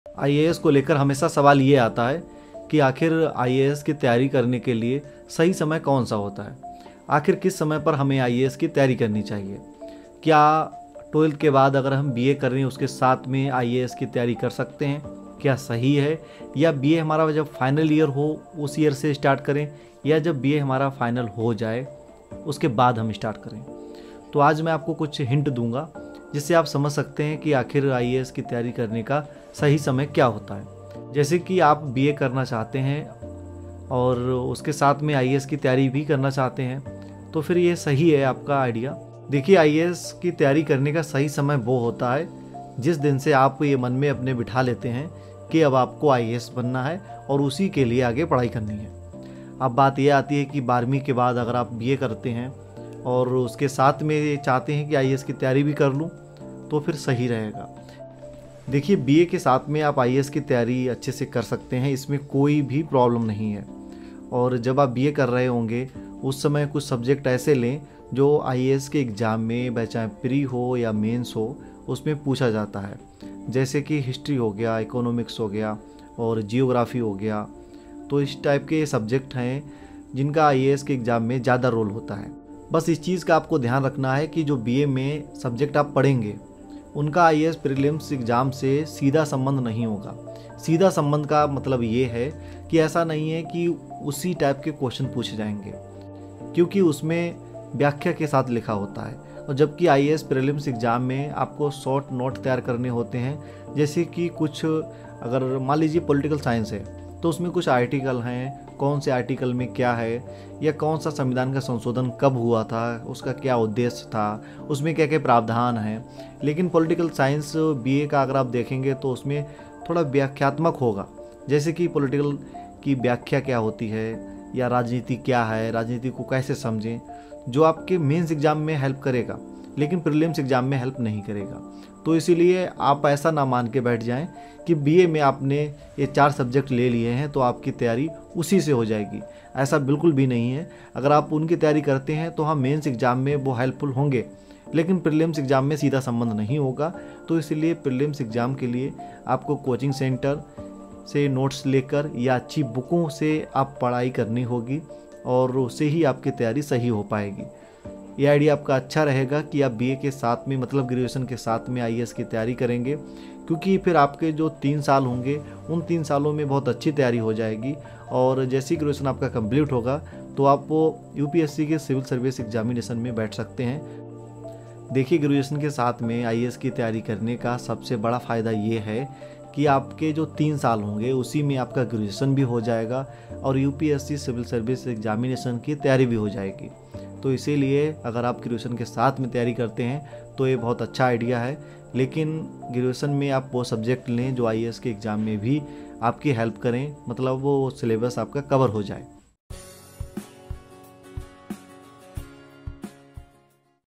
Can I prepare for UPSC with BA || IAS/Collector बनने के लिए BA में Subject’s कौन-कौन से लें
What is the best BA subject for UPSC (IAS/IPS) exams?

It it beneficial to do BA if we to crack IAS?

I'm doing BA and I want to start preparation for IAS

Can I prepare for UPSC with BA?